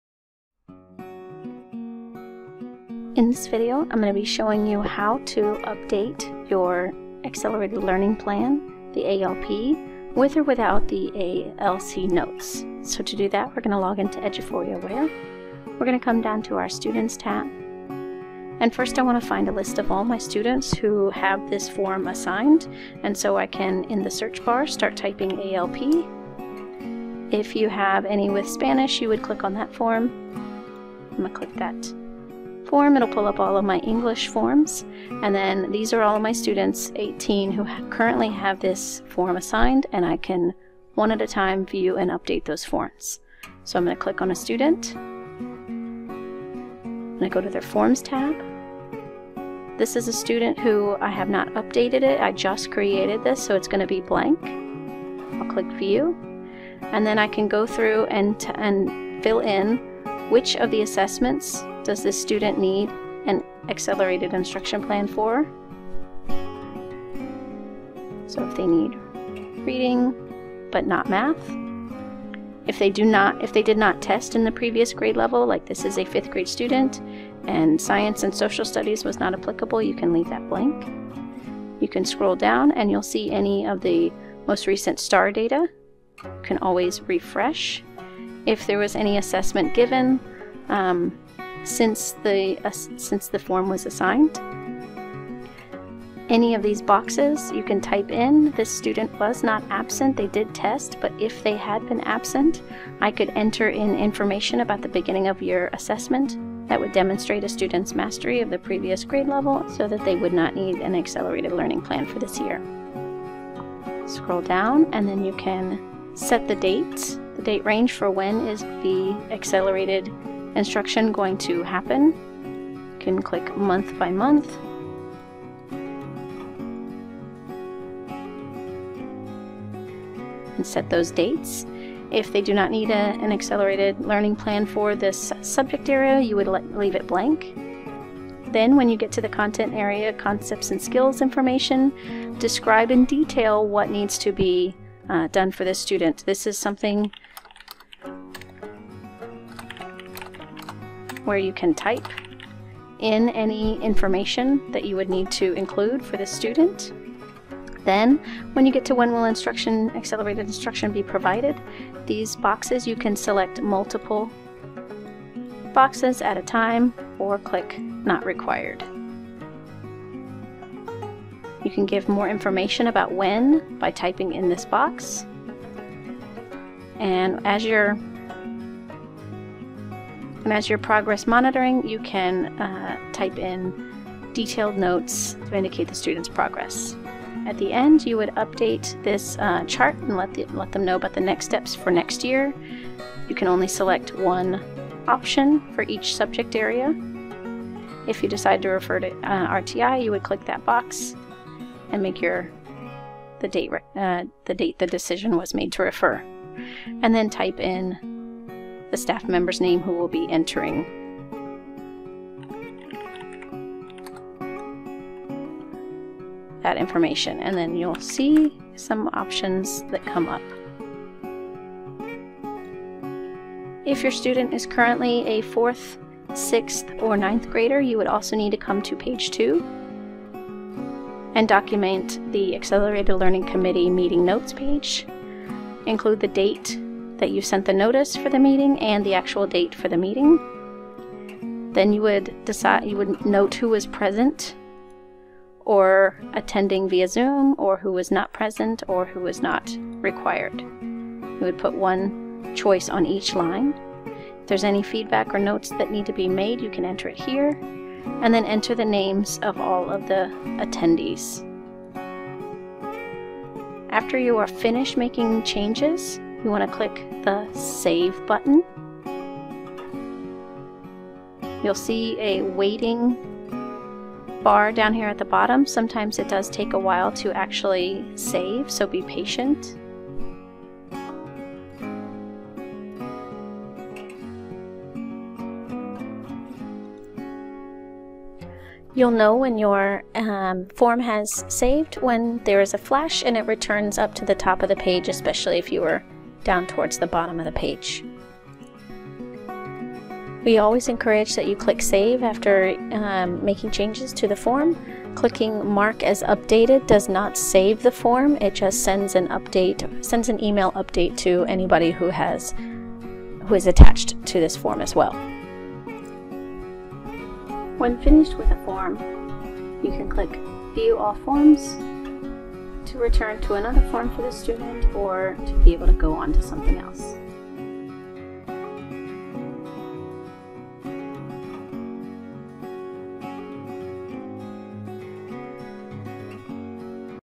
Updating an ALP Form in Eduphoria Aware
In this video for NEISD users only, you will learn how to navigate to and update an Accelerated learning Plan (ALP) form. Depending on the student's grade level, you may or may not need the attached Accelerated Learning Committee (ALC) meeting notes.

Table of Contents:

00:00 - Introduction
00:13 - Navigating to Form
00:55 - Updating Form
03:43 - Gr 4, 6, 9 ALC Meeting Notes
04:22 - Saving ALP Form